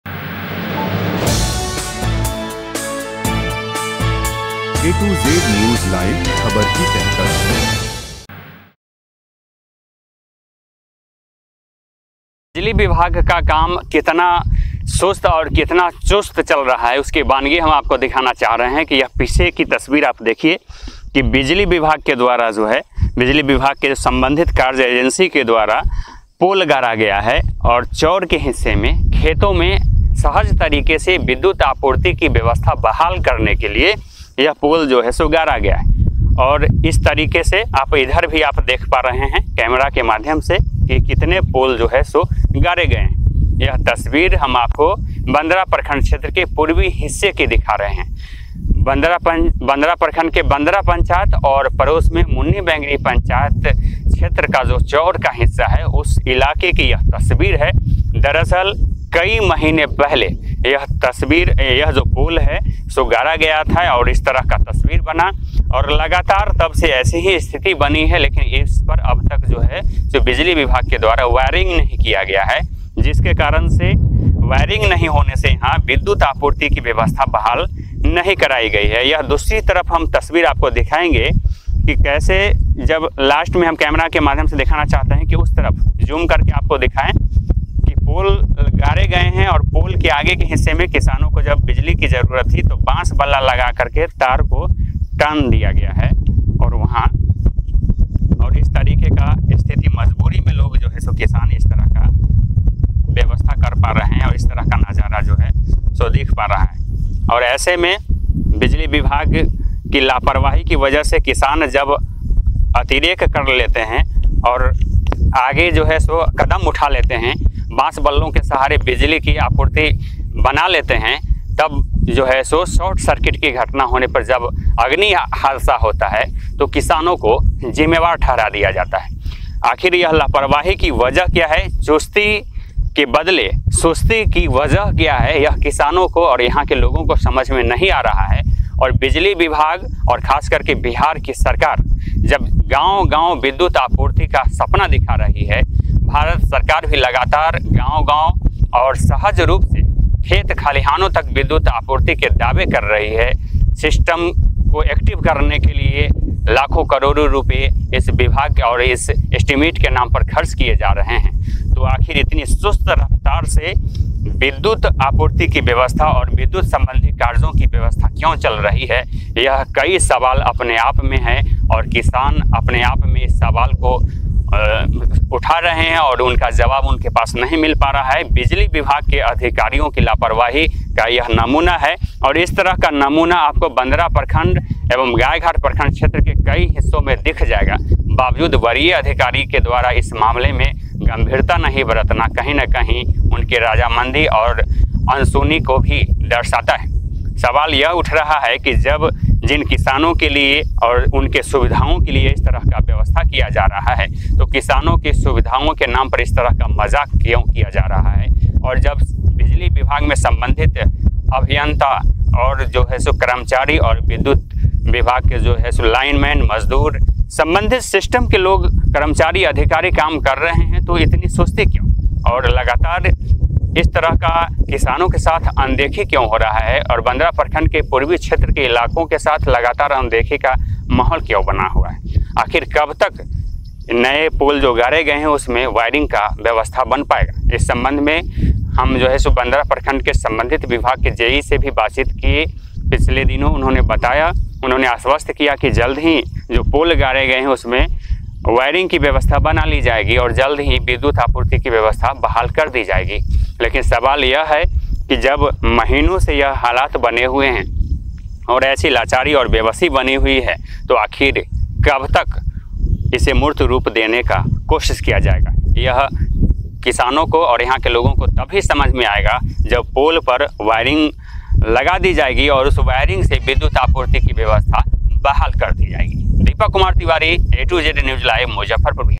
खेतों में सिंचाई के लिए बिजली की किल्लत, ग्रामीणों का गुस्सा फूट पड़ा ⁉️ 👨‍🔧
मुजफ्फरपुर बिहार के बंदरा और मुन्नी पंचायत में बिजली संकट गहरा रहा है। वायरिंग का काम अधूरा होने के कारण ग्रामीणों को भारी परेशानी का सामना करना पड़ रहा है। ⁉️ 👨‍🔧

बिजली विभाग द्वारा खेतों में सिंचाई के लिए निर्बाध बिजली आपूर्ति सुनिश्चित करने के लिए काम शुरू किया गया था, लेकिन काम में अनावश्यक देरी हो रही है। ग्रामीणों का आरोप है कि विभाग इस समस्या को नजरअंदाज कर रहा है। बार-बार शिकायत करने के बाद भी विभाग की तरफ से कोई ठोस कदम नहीं उठाया गया है।

बांस के खंभों पर झूलते खतरनाक तार, ग्रामीणों की सुरक्षा पर सवाल!

मौजूदा समय में, मजदूरों द्वारा बांस के खंभों पर तार बिछाकर बिजली आपूर्ति की जा रही है, जो कि सुरक्षा के लिहाज से भी बेहद खतरनाक है। ग्रामीणों का कहना है कि विभाग उनकी सुरक्षा को लेकर लापरवाह है।

ग्रामीणों ने की जल्द समाधान की मांग, बिजली विभाग पर बढ़ा दबाव!

इस स्थिति से ग्रामीणों में भारी रोष व्याप्त है। उनका कहना है कि विभाग को जल्द से जल्द वायरिंग का काम पूरा कराना चाहिए ताकि नियमित बिजली आपूर्ति बहाल हो सके।

उम्मीद की जाती है कि बिजली विभाग इस समस्या का जल्द समाधान करेगा और ग्रामीणों को निर्बाध बिजली आपूर्ति सुनिश्चित करेगा।